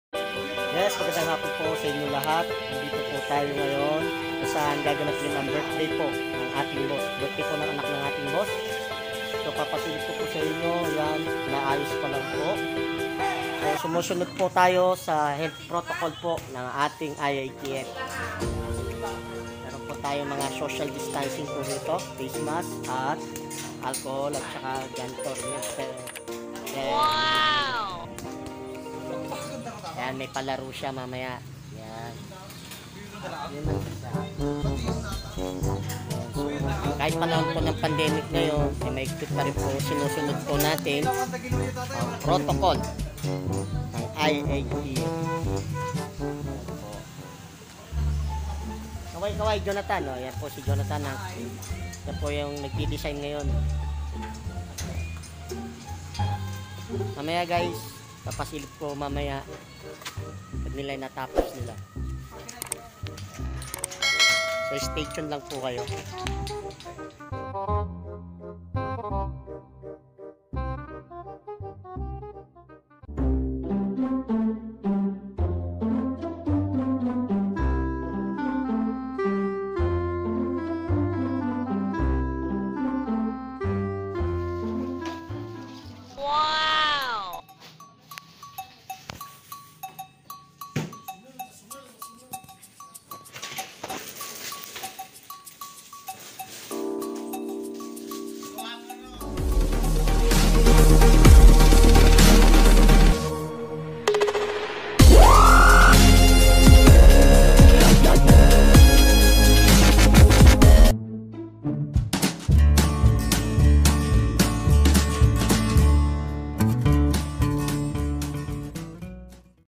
BIRTHDAY CELEBRATE WITH DISKARTENG BARBERO 🎈🎂
hello guys it's birthday celebration nakakamis Yung ganitong okasyun Sana matapos na pandemic para namn ma enjoy natin laha hopefully masama NYO ako magdarasal na bumalik na Yung normal natin ..pls  support my channel God bless us..FB PAGE MR BARBERSHOP